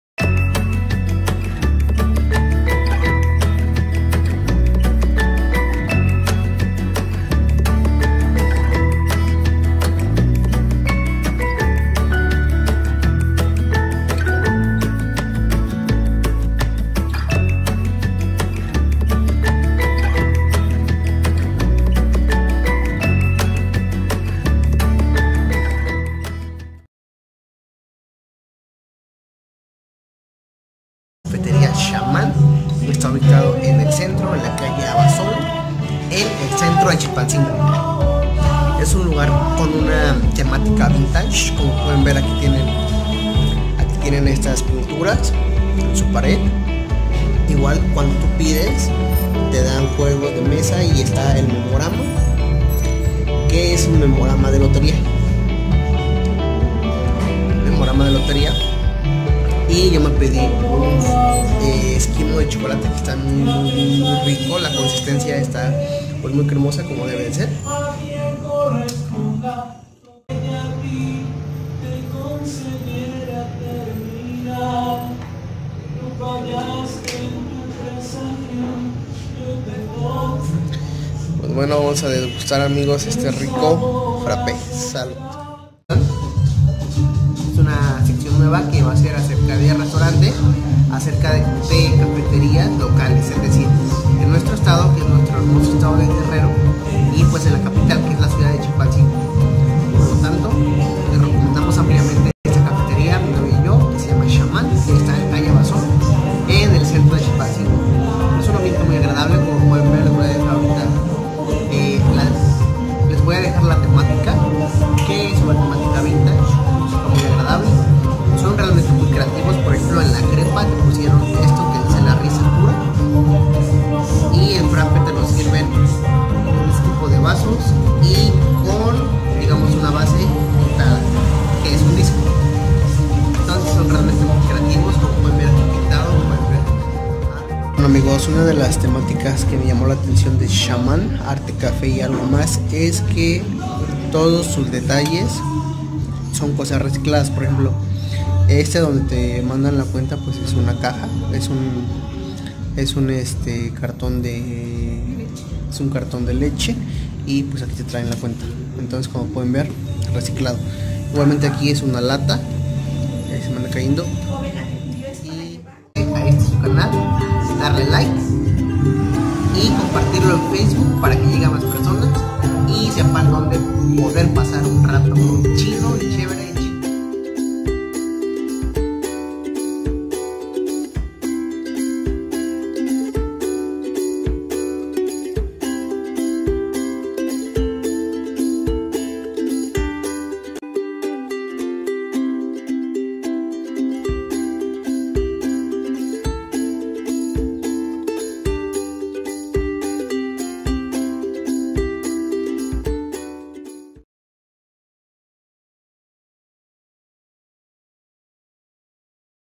🔥VISITANDO XAMAN CAFÉ EN CHILPANCINGO 🔴/ EL YIZUZ BLOGGER
En este vídeo les muestro una gran opción sí están en la ciudad de Chilpancingo y quieren visitar una Cafetería de calidad, excelente sabor y muy buenos precios, les habló acerca de Xaman: Café, arte y algo más. ☕🥧
Puedes disfrutar desde un rollito xaman por solo $8 cada uno, hasta un delicioso eskimo de chilate, en verdad les recomiendo este gran lugar para pasar una tarde muy amena.

🔴MILLONES DE GRACIAS GENTE BONITA DE YOUTUBE🔴